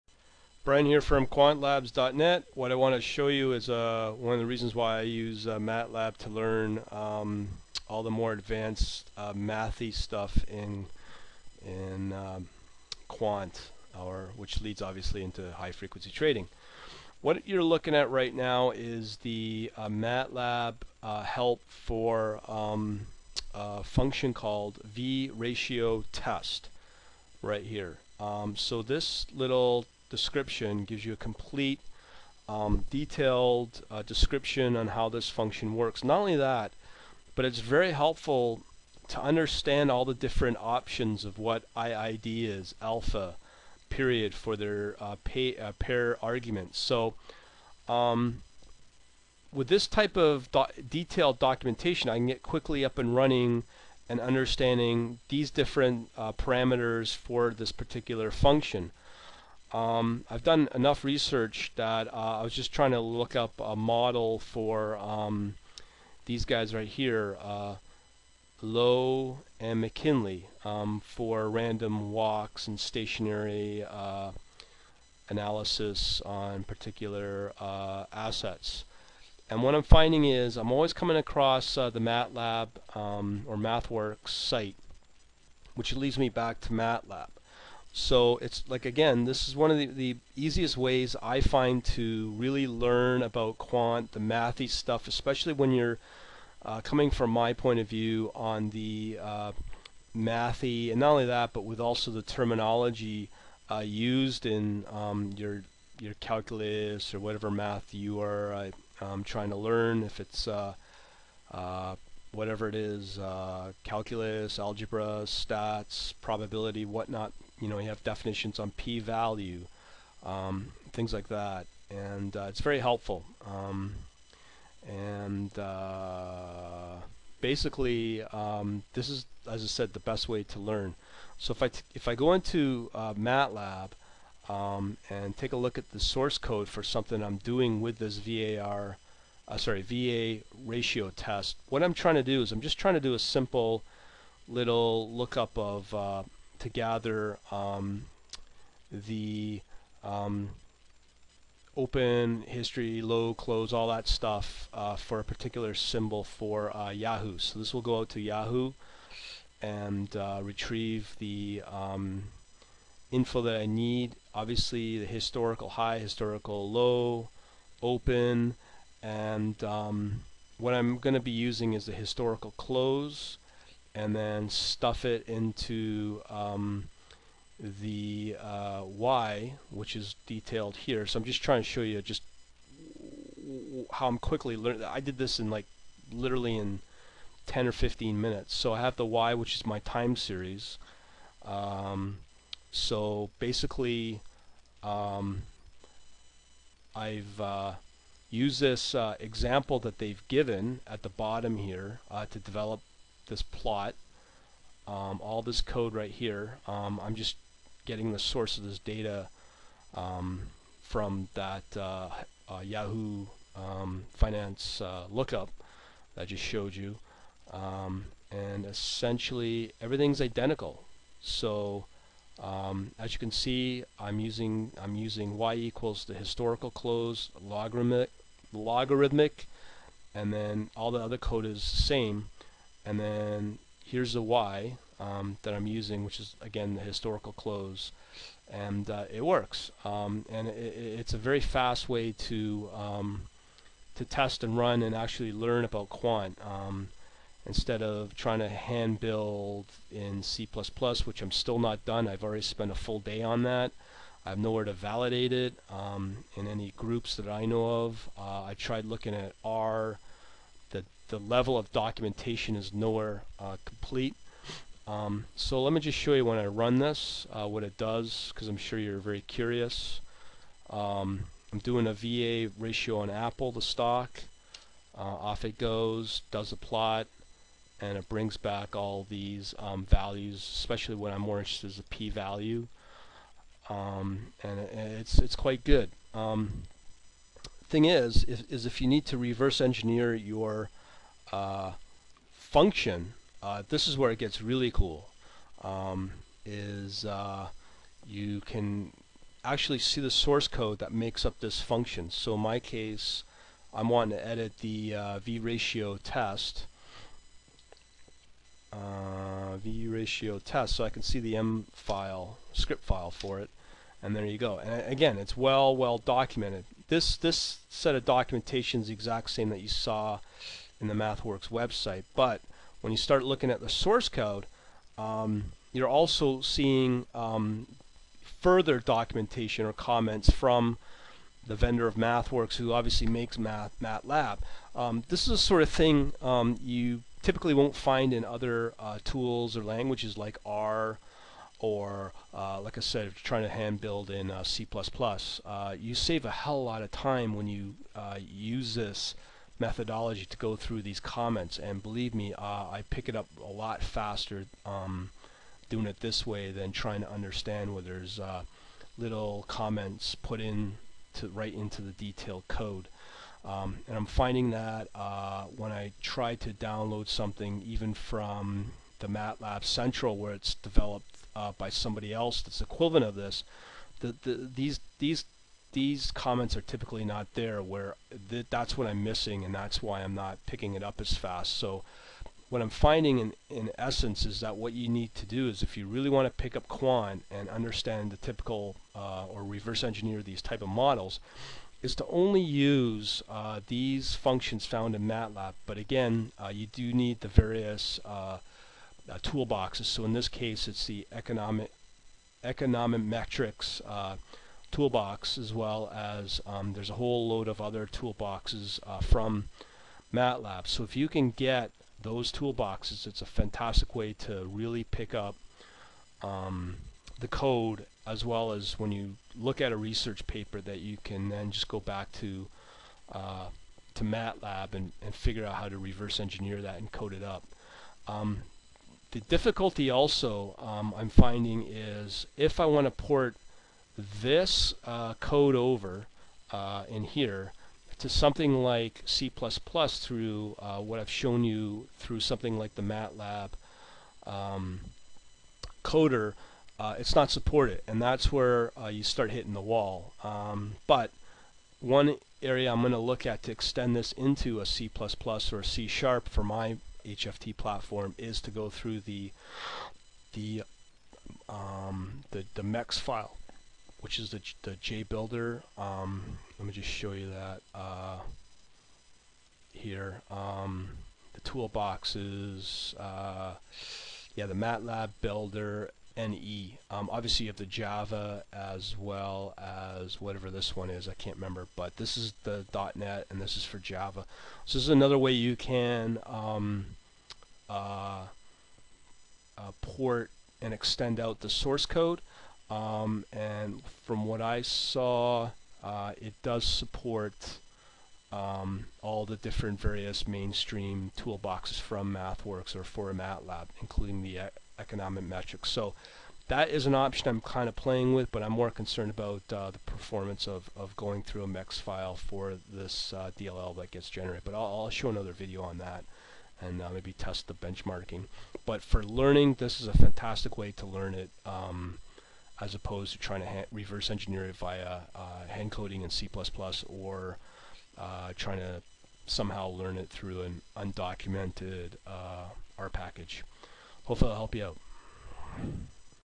Learn quant models and algos FAST for HFT through Matlab
I show why I use Matlab to quickly learn models and algos. This helps me quickly reverse engineer quality Matlab functions that are properly commented/documented from Mathworks. This is instead of pulling your hair out with poorly undocumented R packages or hand building your own C++ models.